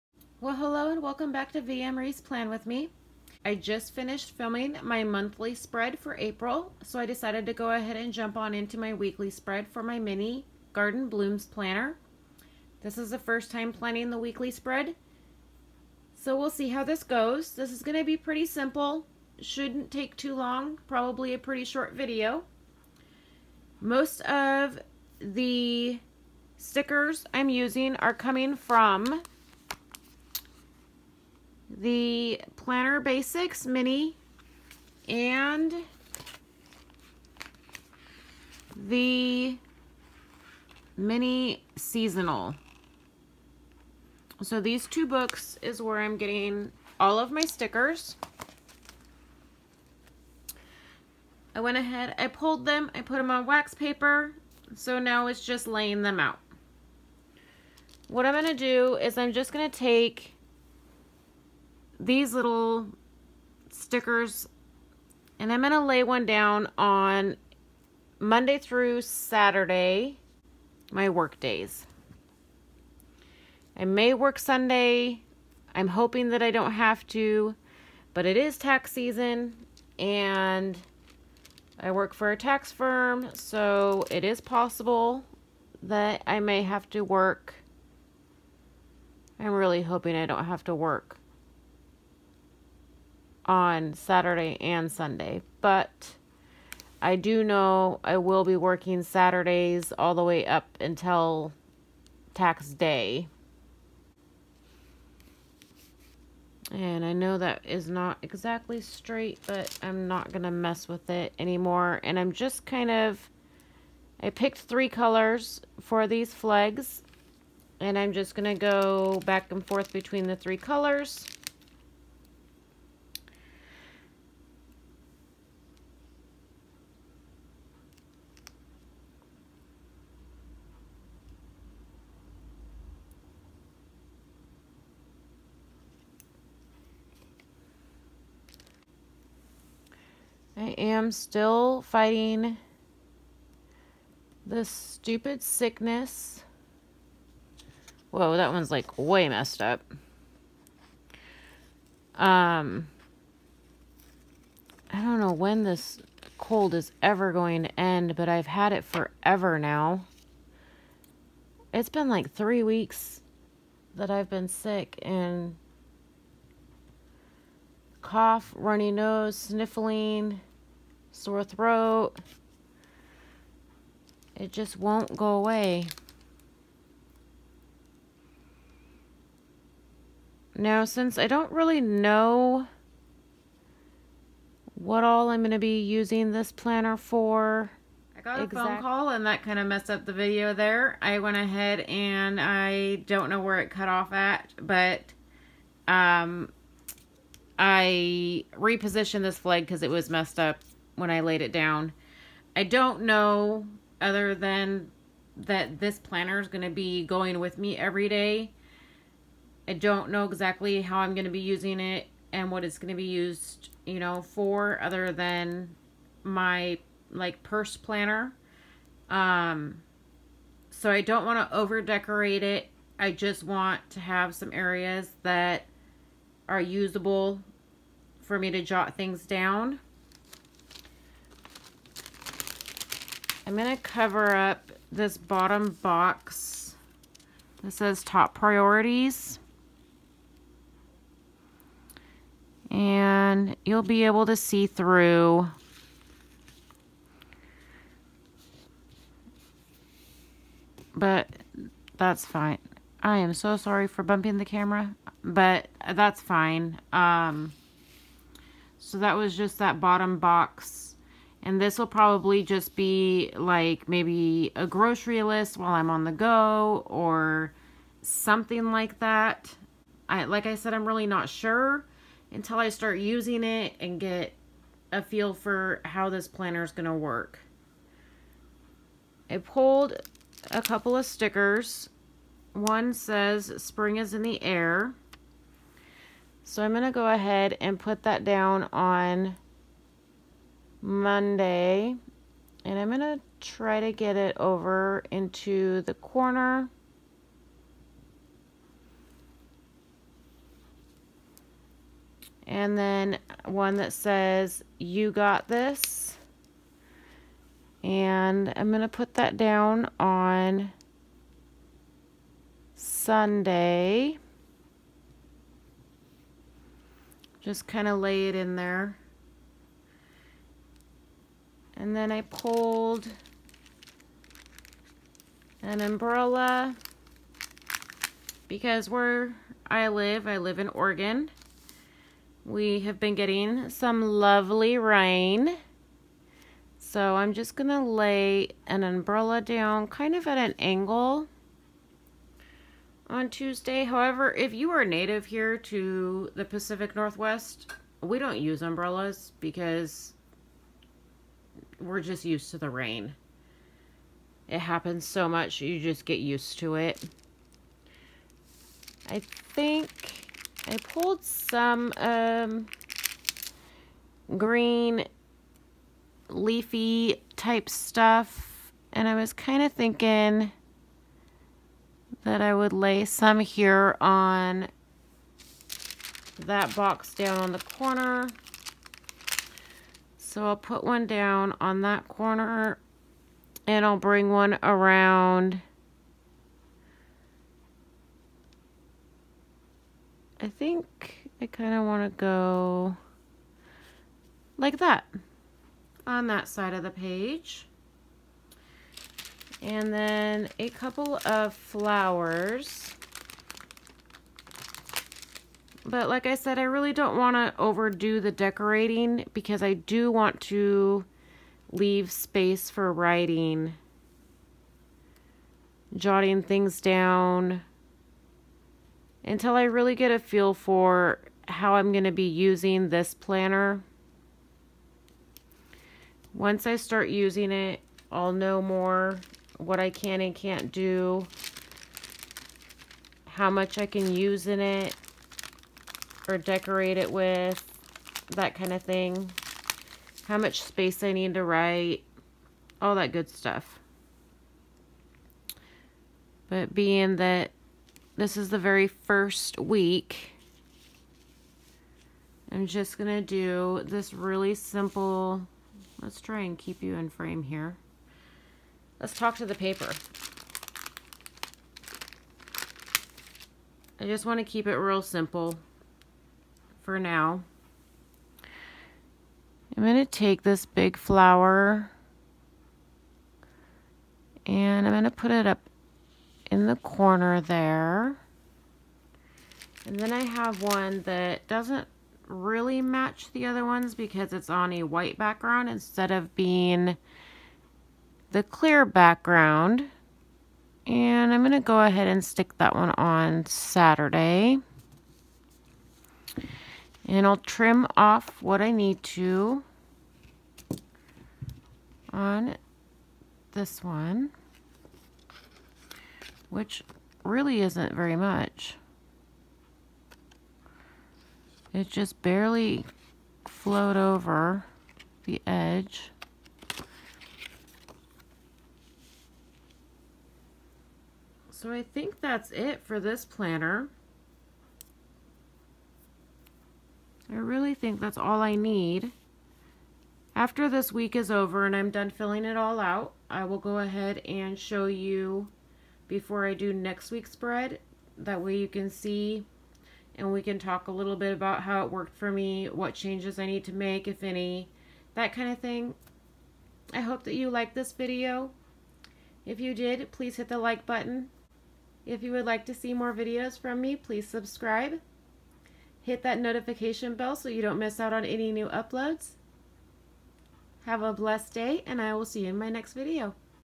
Here it is... My Very First Weekly Spread in the Happy Planner Mini!
Welcome and thank you for joining me today! This is my first time planning in my mini horizontal layout Garden Blooms Planner.

I don’t really know how this is going to work for me, I do know it’s my on the go Planner aka purse planner, so I did a simple layout with little decor so I can get a feel for how I’ll be using this Planner.